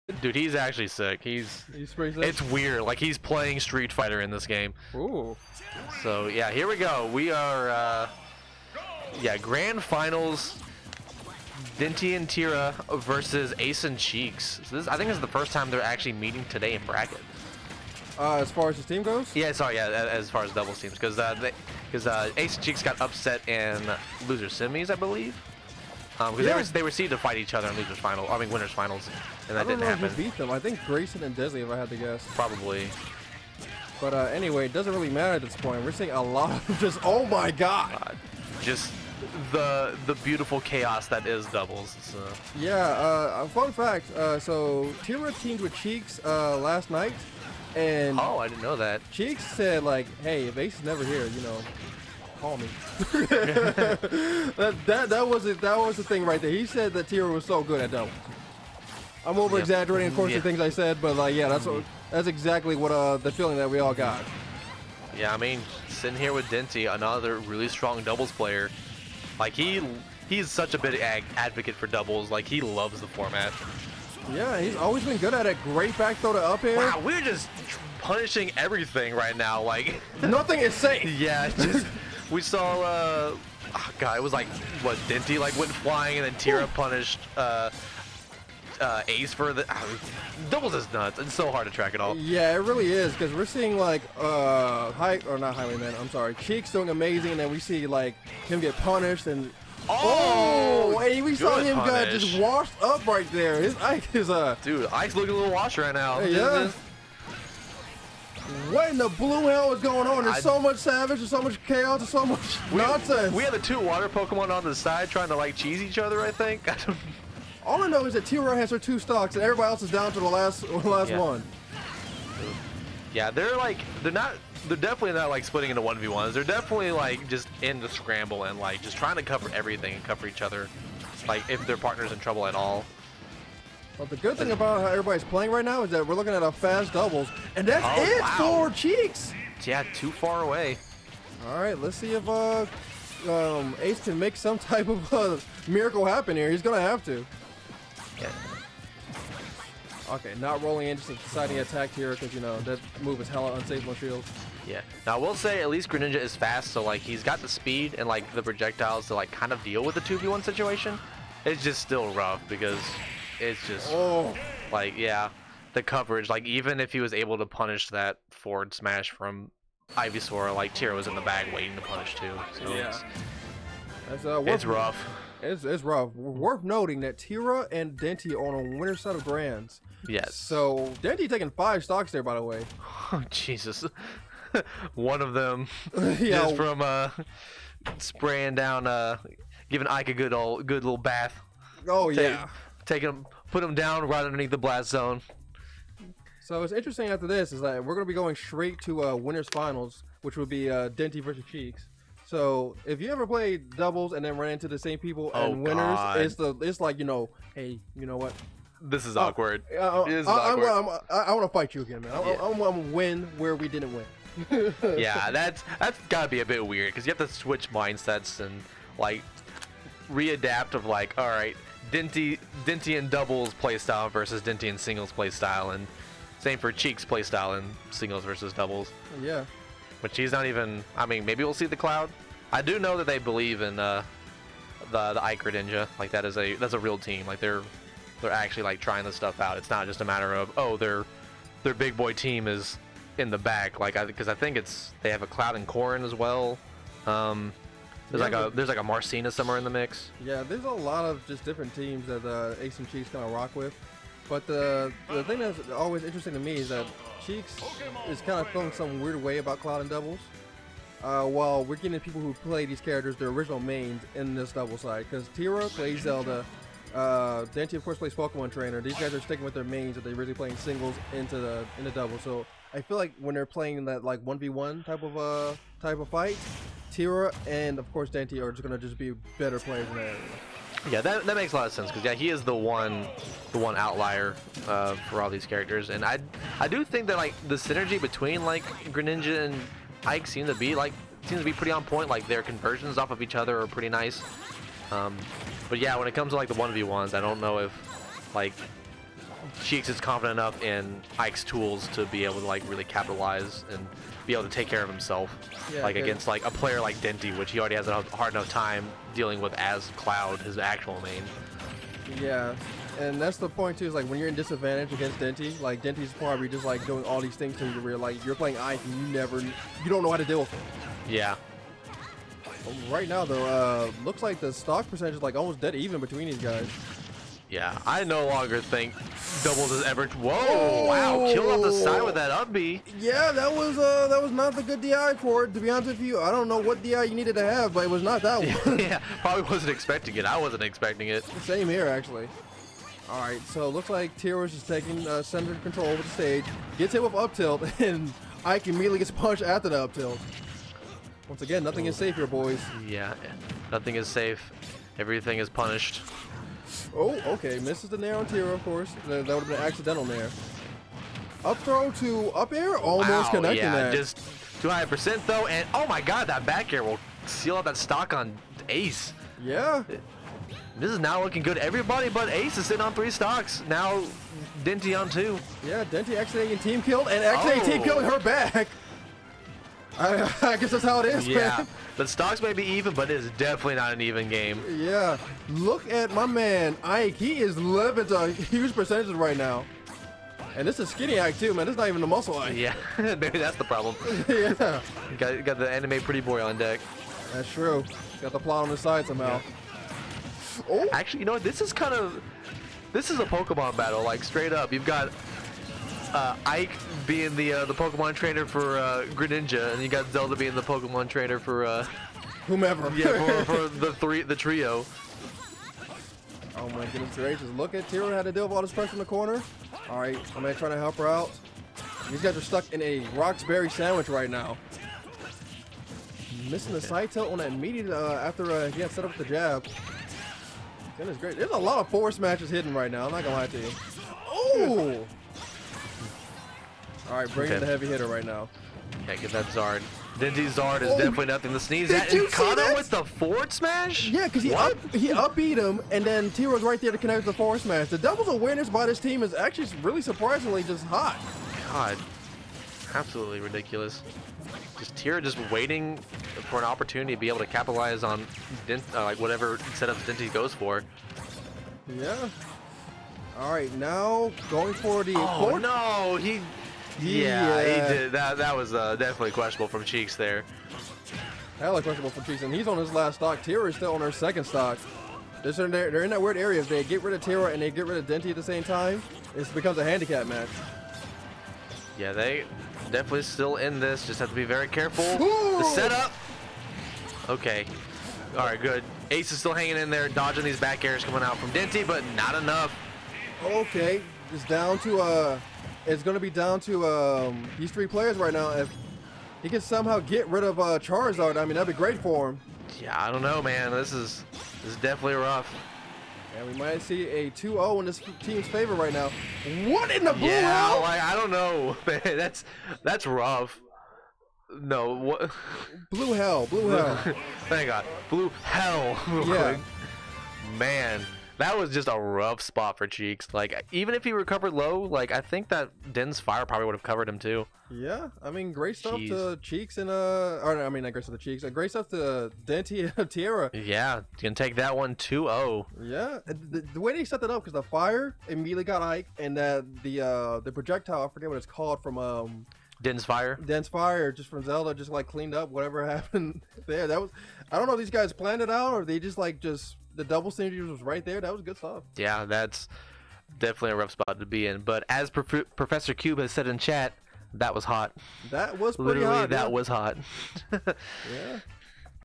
FREAKS Ultra 60 Doubles Grand Finals: Denti & Tira [W] vs ASE & Cheeks [L]
FREAKS Ultra 60 Doubles Grand Finals: Denti & Tira [W] vs ASE & Cheeks [L] -- Watch live